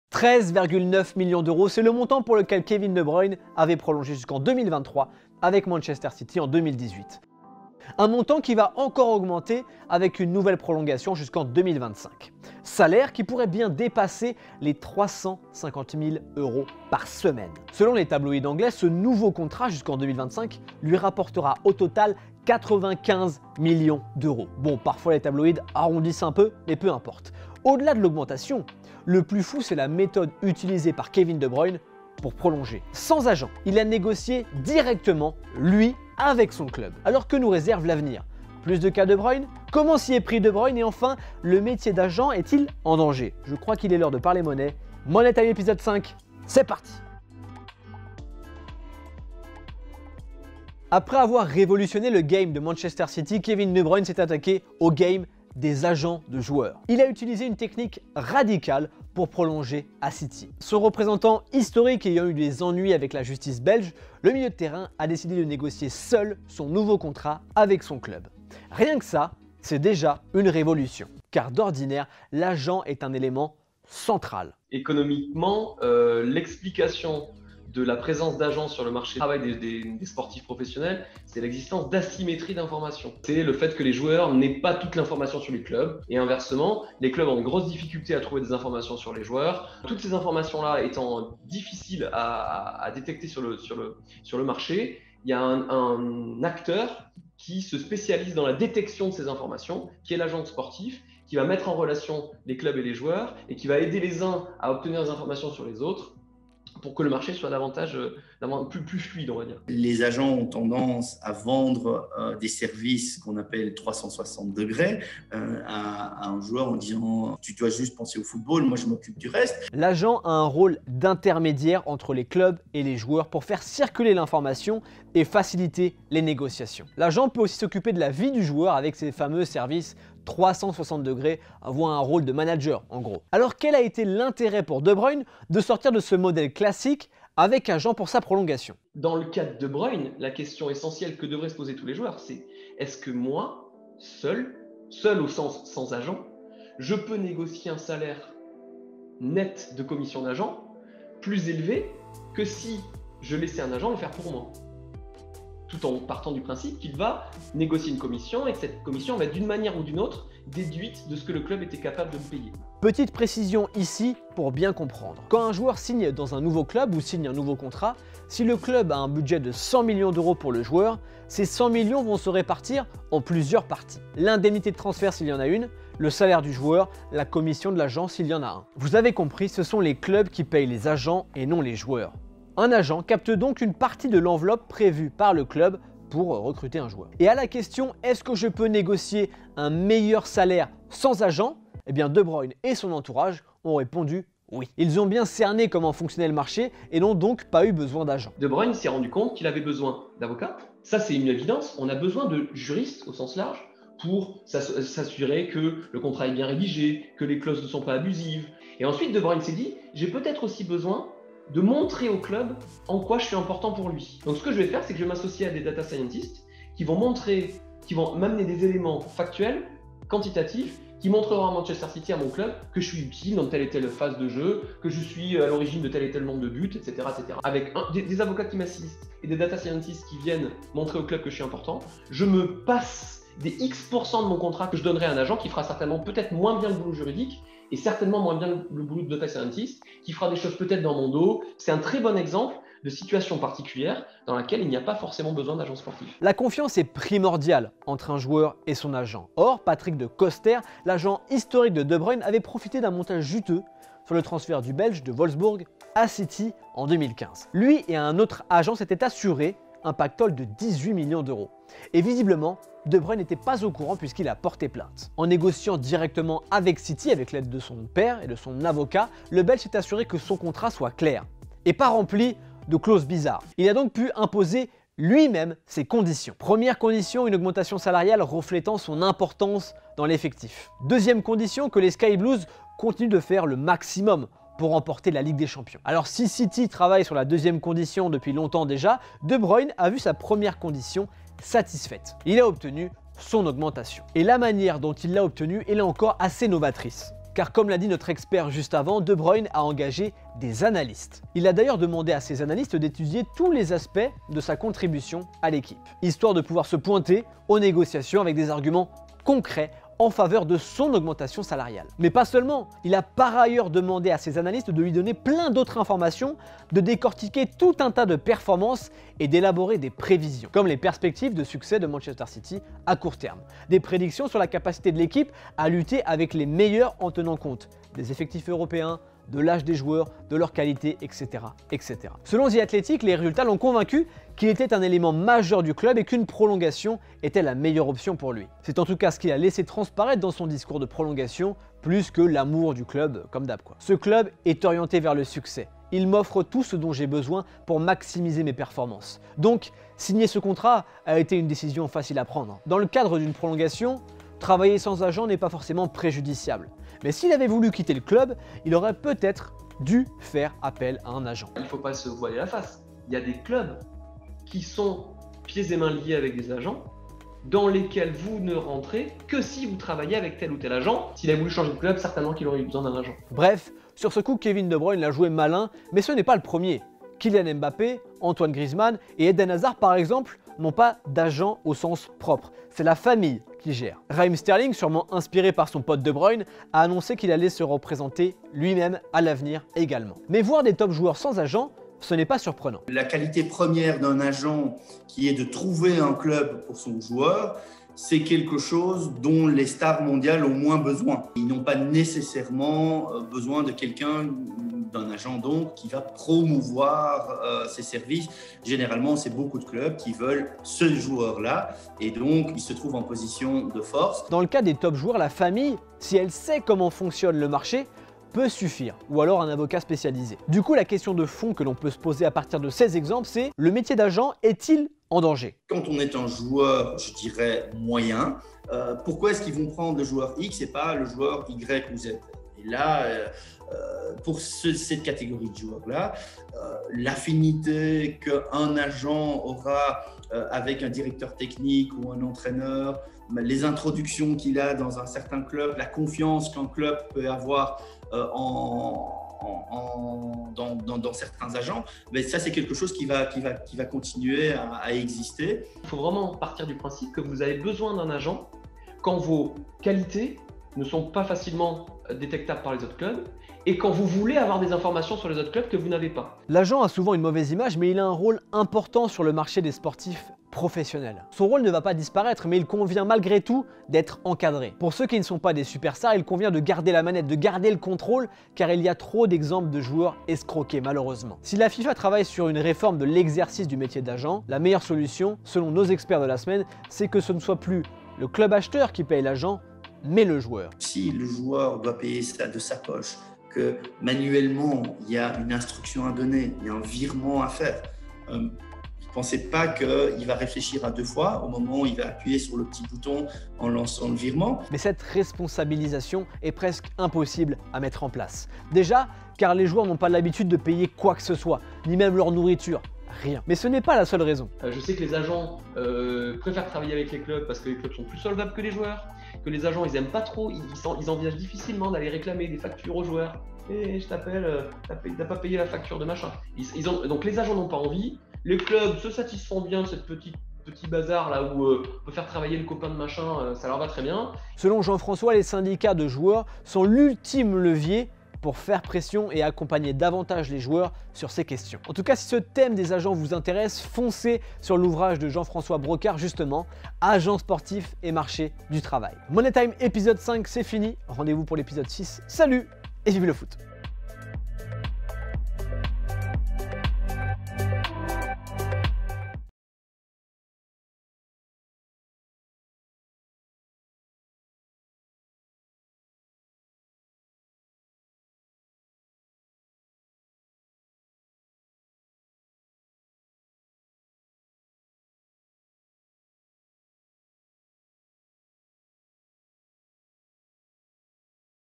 La technique radicale de Kevin De Bruyne pour obtenir un contrat à 95M€ sans agent | Money Time
Kevin De Bruyne a prolongé jusqu’en 2025 à Manchester City. Selon les tabloïds, ce nouveau contrat lui rapportera 95M€ sur 4 ans ! Et pour prolonger, il a utilisé une technique radicale ! Pas d’agent, des datas analystes, des conditions claires, pas de clauses étranges : une méthode très novatrice ! Alors comment s’y est pris Kevin De Bruyne ? Le métier d’agent est-il en danger ?! On essaie de répondre à ces questions dans le 5e épisode de Money Time ! Le show éco d’Oh My Goal !